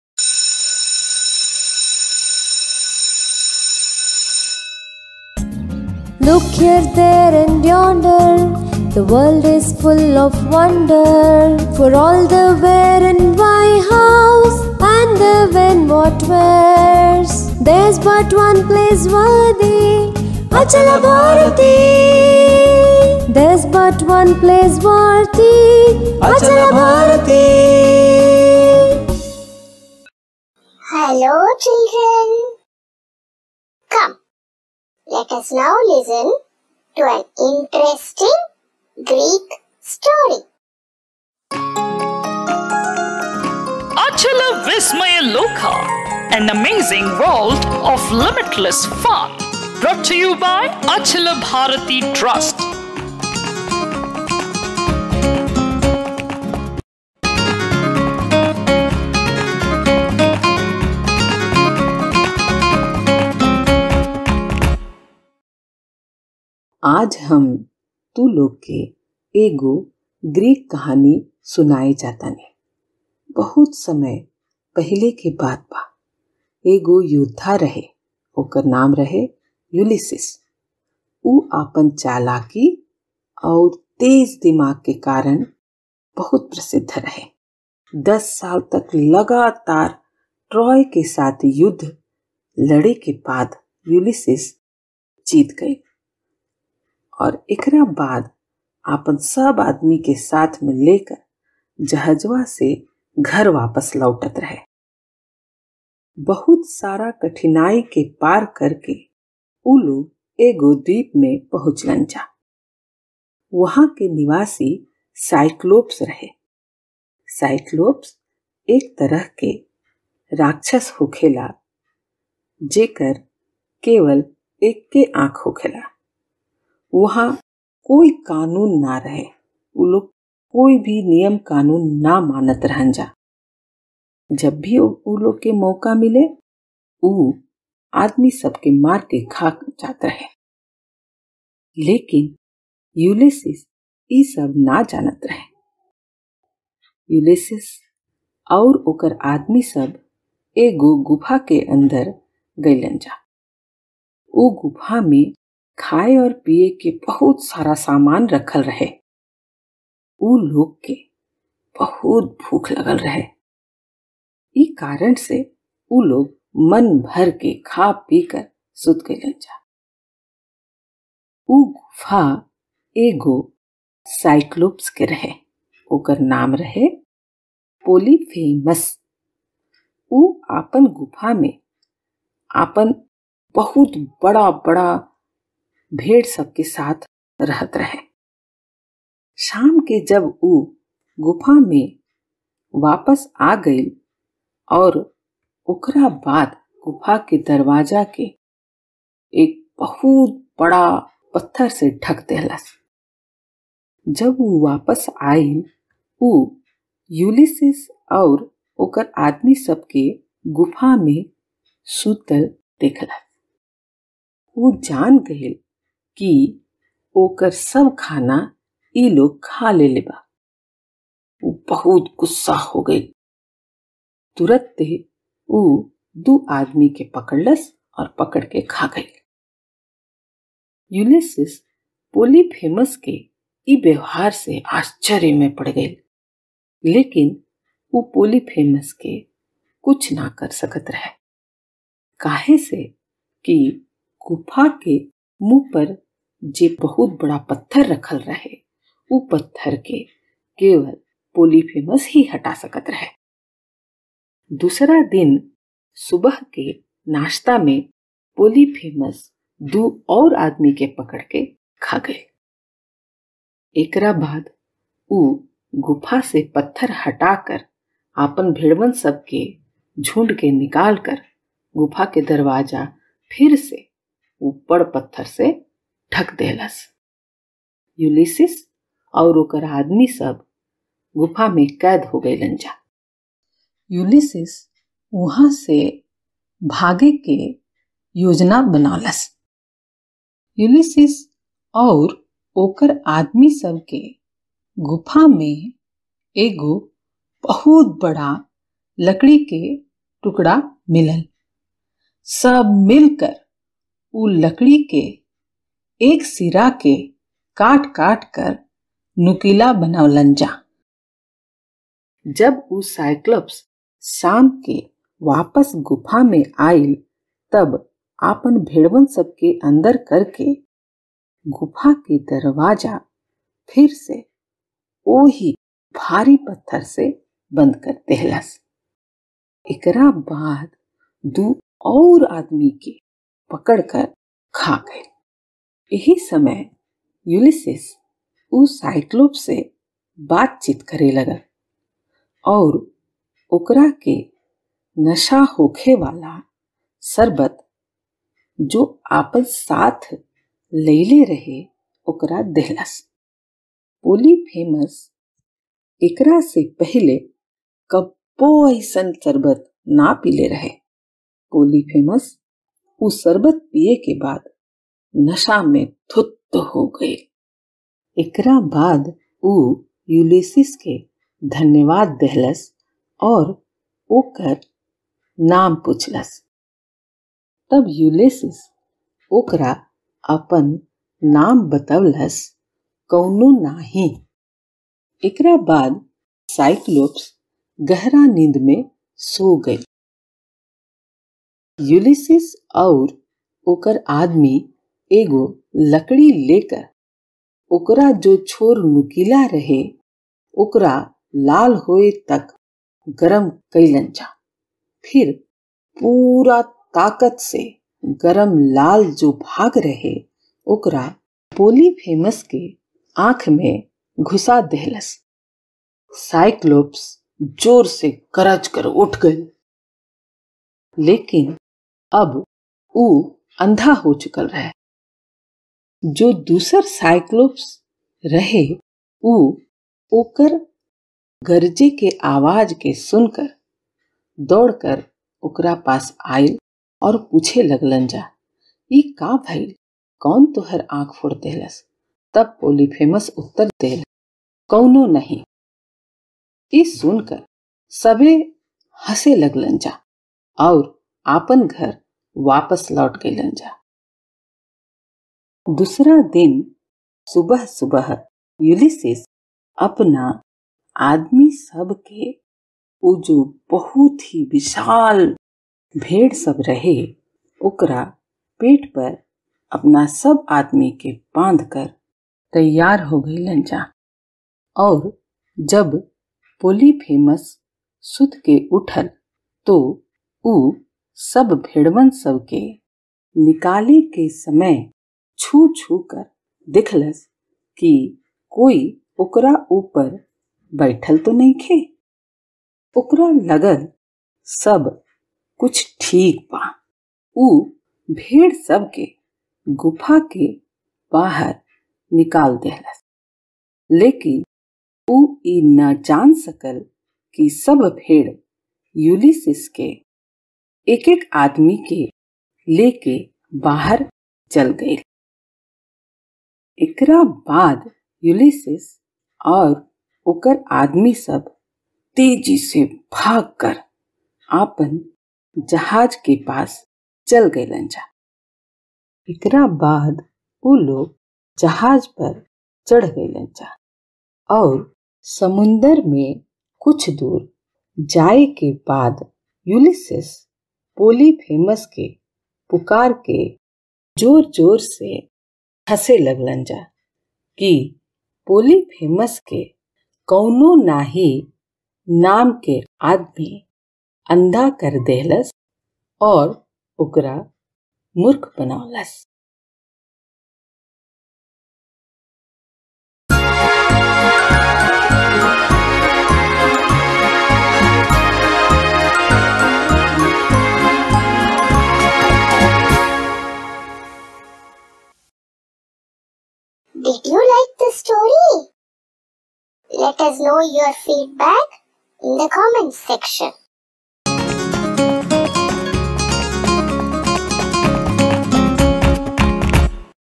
Ulysses And Cyclops – भोजपुरी
यूलिसिस अउर साइक्लोप्स – भोजपुरी

Here is an interesting Greek story. It is about the Greek hero Ulysses who, through his cleverness, escaped from the clutches of the giant Polyphemus, in the land of the Cyclops.

Narrated By: Mahima Verma
Illustrations By: Paneendra Suresh